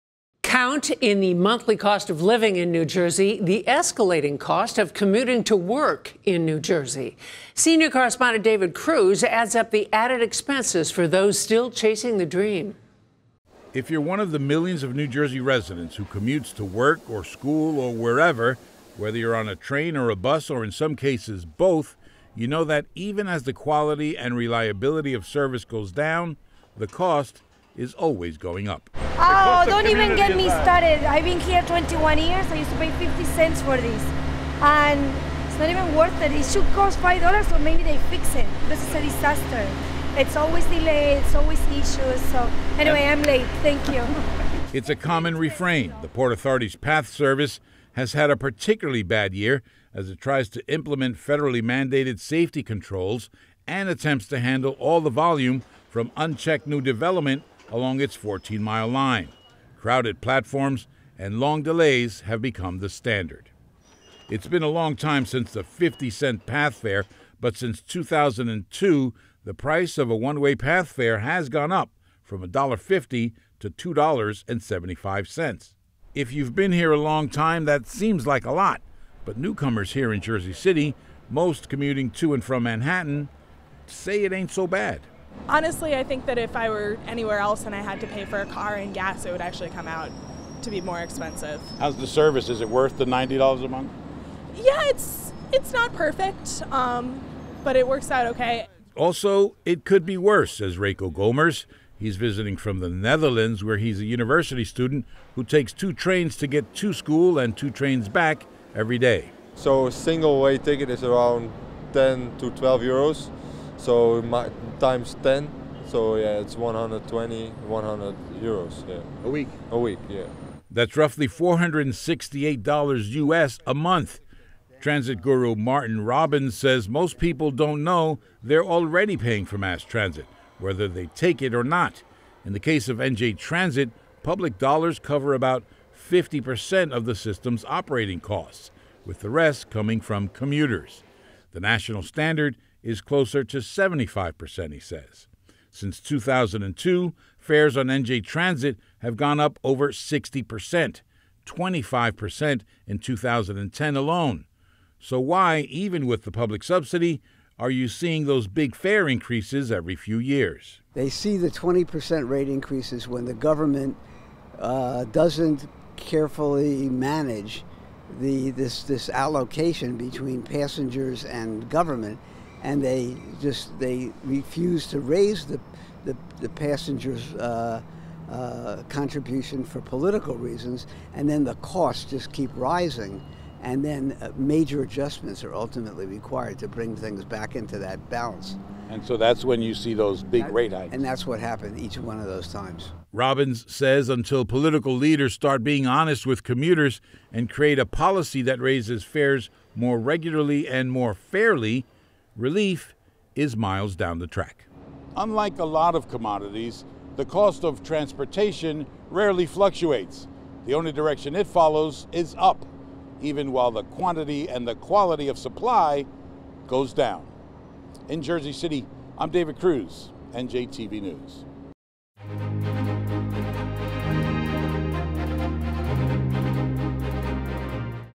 Transportation costs keep rising while service continues to decline
Commuters are paying more and still getting less.